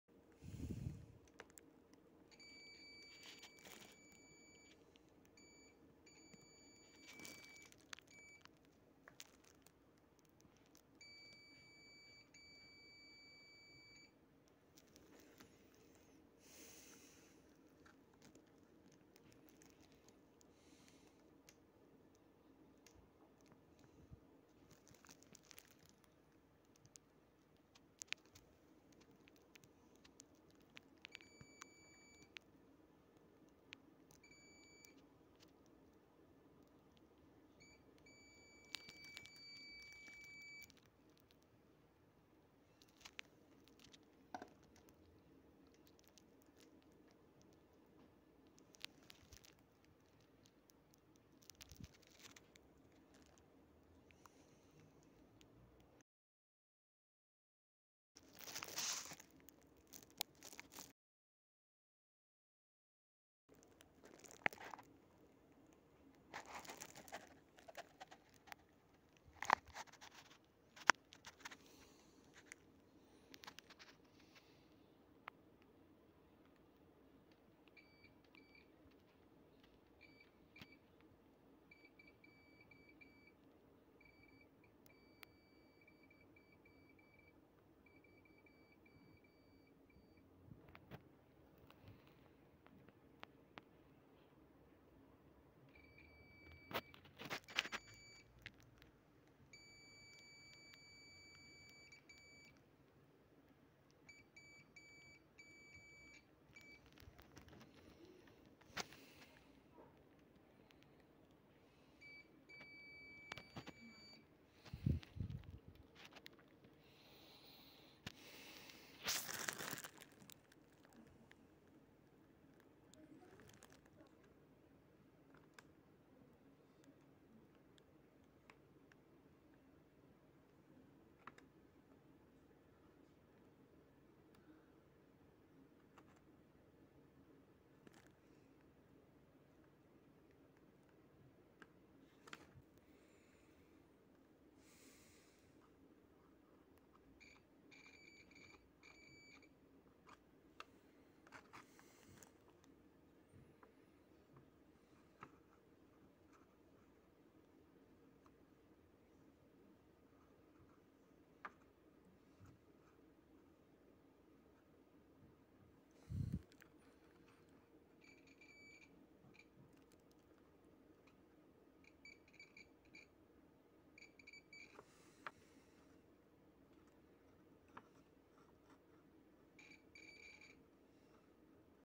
Keyboard trace matrix repair ,chinese conductive paste review and testing.
Bought some conductive paste on aliexpress and wanted to film it and give a visual review of the resistance. copy paste the url, cause youtube wants my phone number and I aint giving it.